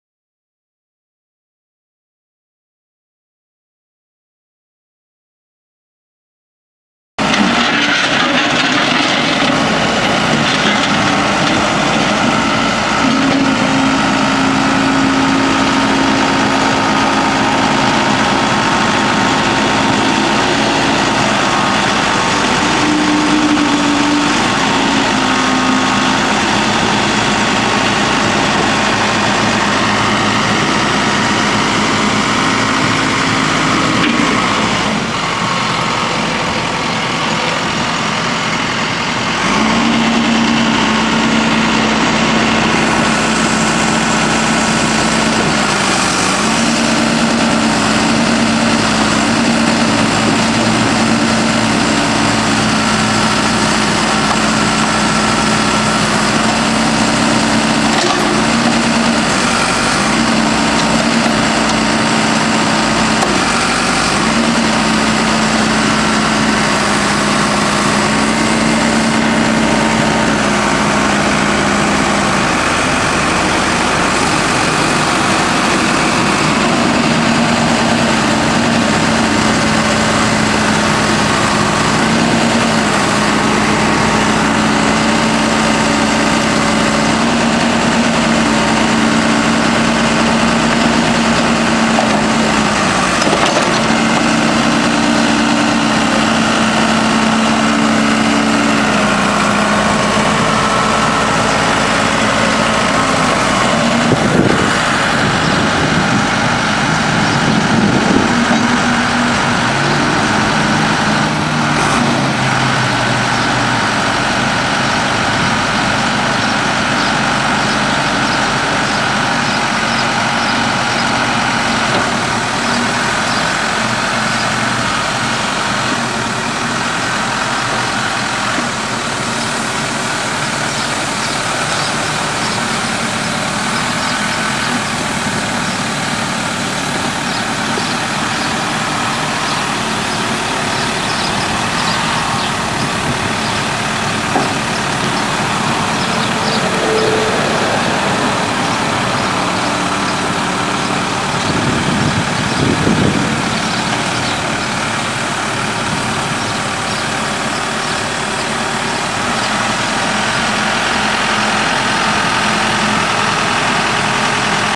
1998 Case 580 Super L backhoe for sale | sold at auction September 3, 2015
SOLD! Manhattan Pipeline LLC Equipment Auction

1998 Case 580 Super L backhoe
6,192 hours on meter
Case four cylinder diesel engine
Shuttle shift transmission
4F-4R gears
Four wheel drive
OROPS
Serial JAB0156043
Wobble stick backhoe controls
82"W loader bucket
Extendable stick backhoe
24"W backhoe bucket
12 x 16.5 front tires
19.5L-24 rear tires
13,462 lbs. base operating weight
Serial JJG0204963
Unit# 53-14